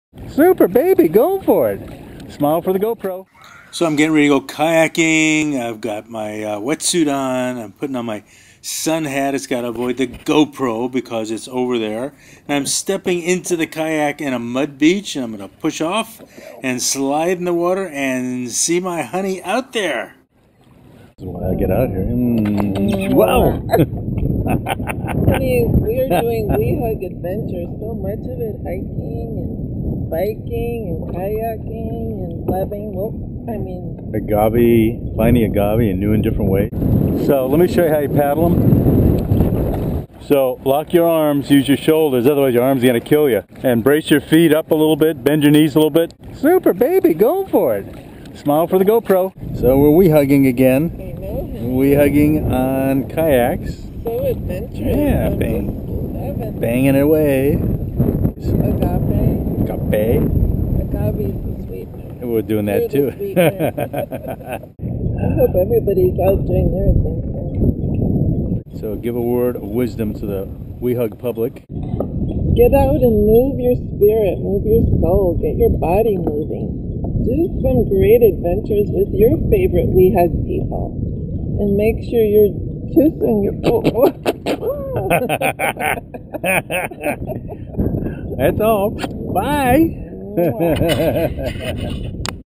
GoPro straped to my head while kayaking in Marina de Rey, CA
Youtube GoPro video  kayaking in Marina del Rey, CA. GoPro attached to my head via the GoPro head band accessories. I Love my GoPro 2 Hero!!

"Today is a great day to buy a yacht"

Randall Burg